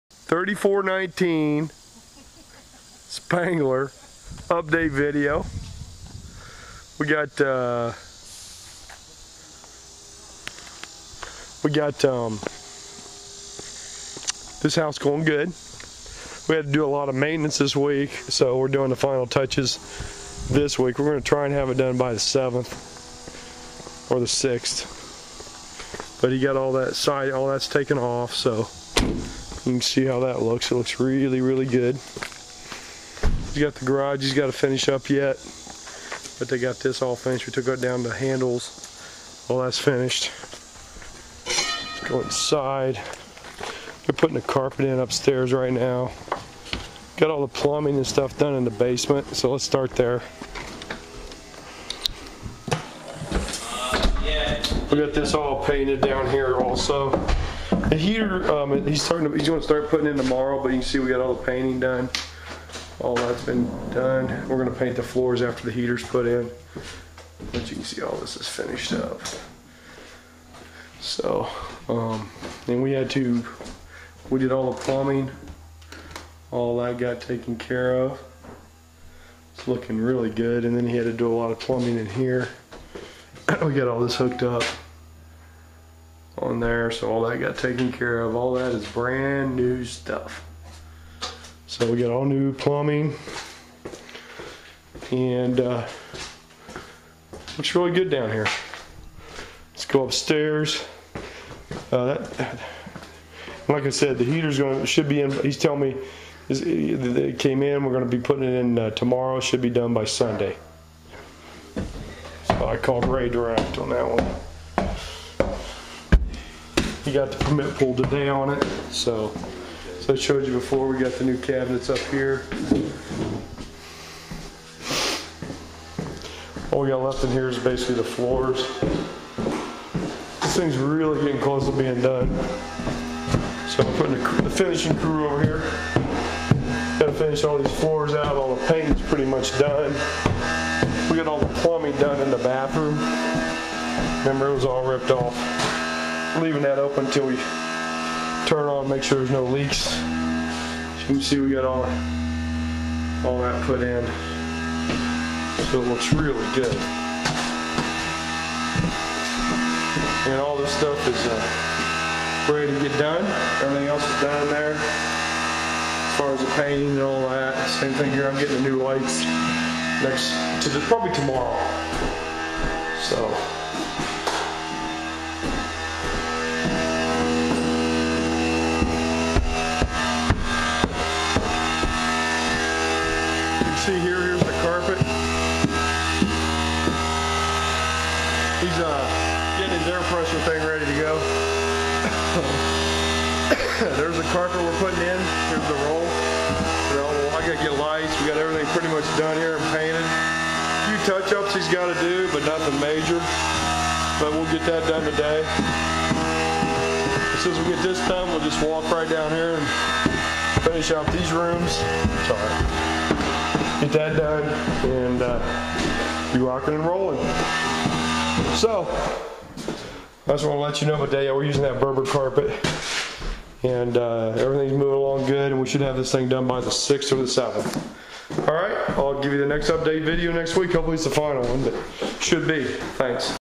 3419 Spangler Update
Great 4 Bedroom 1 bath Home large Lot Double Car garage